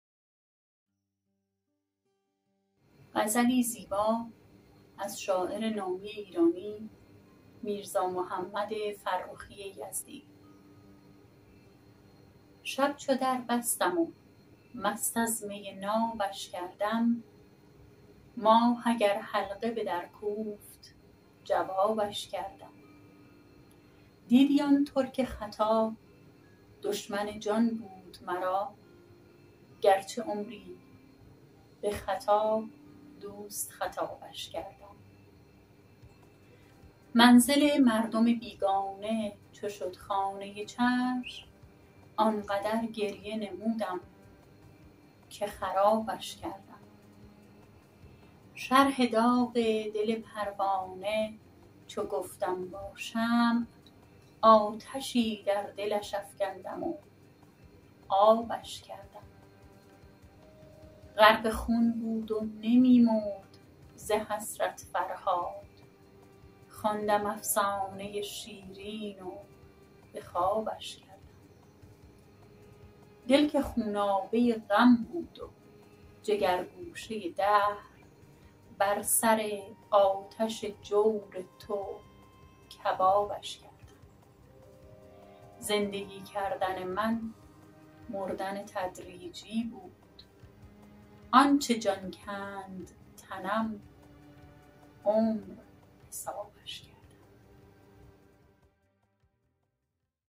غزلی از محمد فرخی یزدی
A poem by Mohammad Farrokhi Yazdi

مجری: سوسن جعفرزاده
Host: Susan Jafarzade

آهنگساز:
حامد سلطانی
Composer:
Hamed Soltani